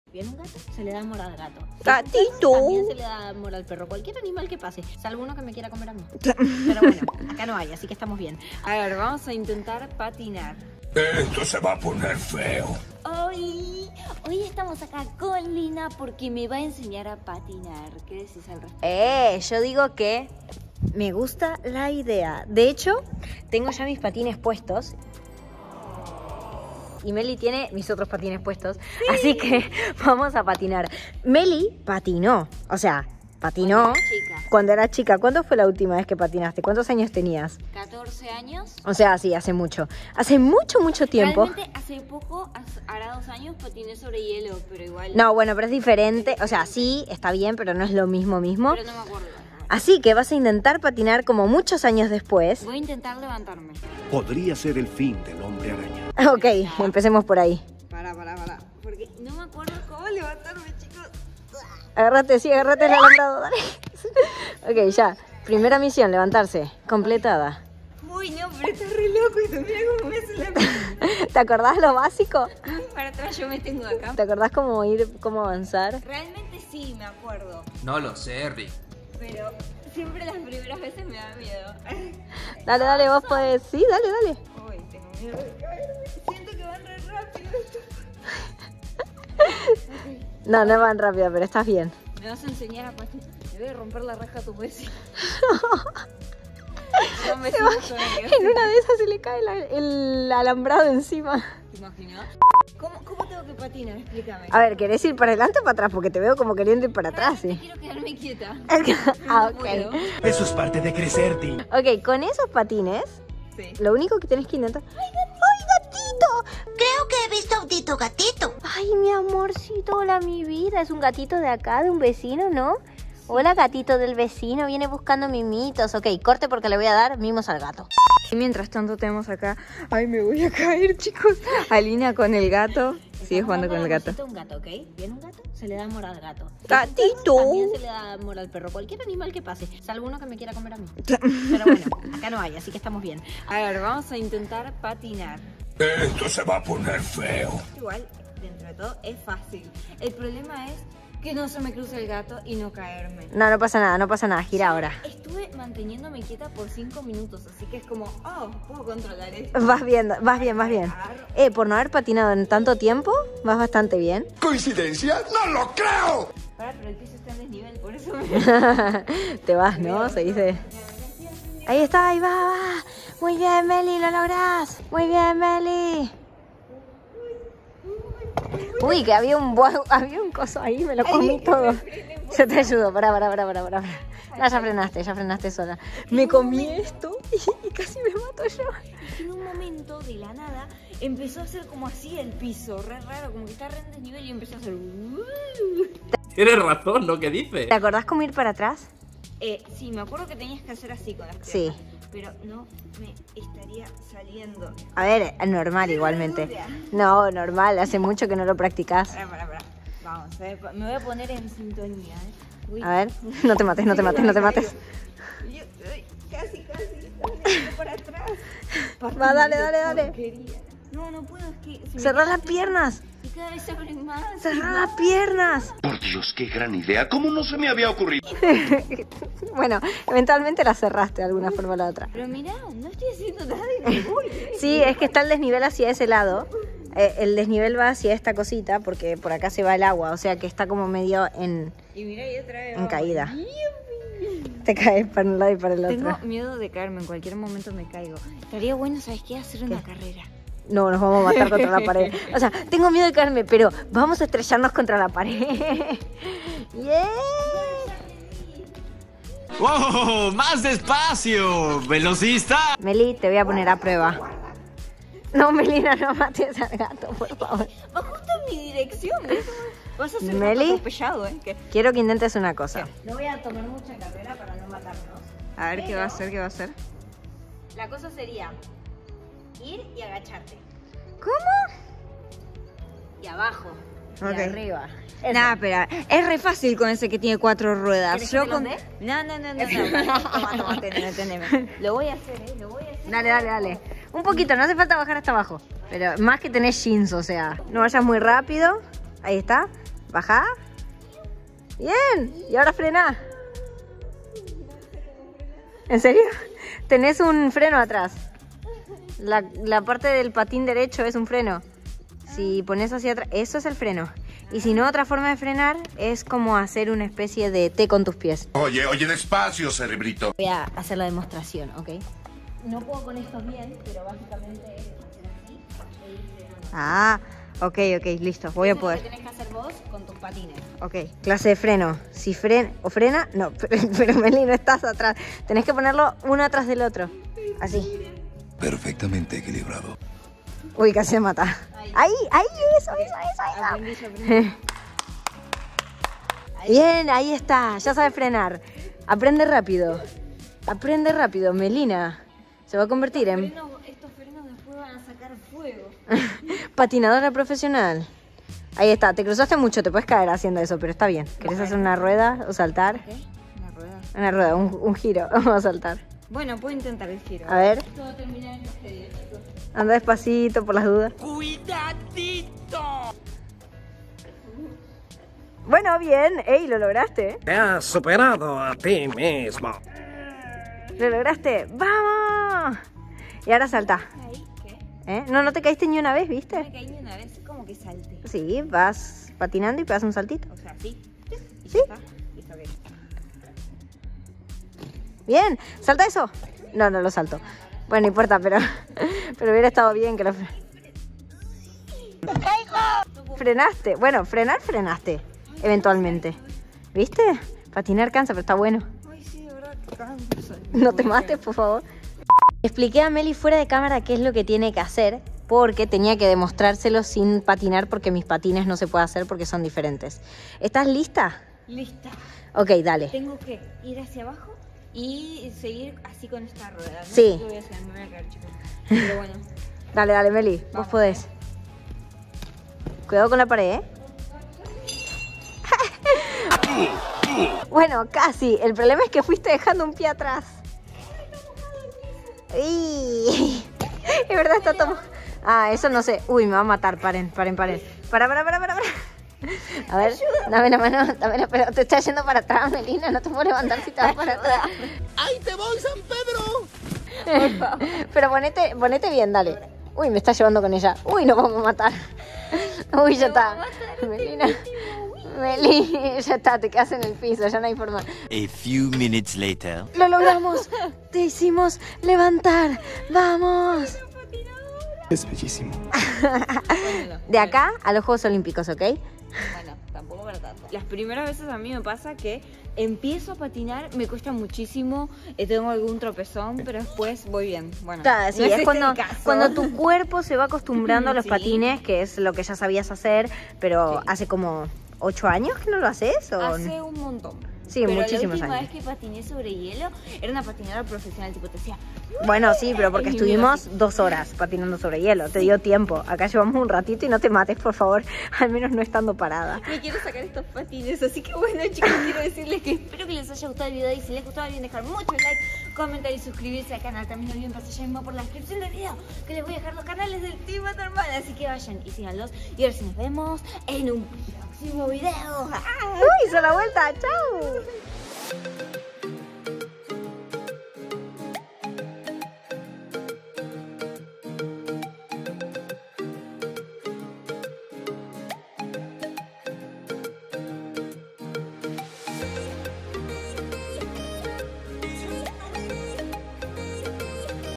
LYNA ME ENSEÑÓ A PATINAR Y ESTO PASÓ
Hola gente de Youtube! Me llamo Melina y en el video de hoy LYNA ME ENSEÑARÁ A PATINAR!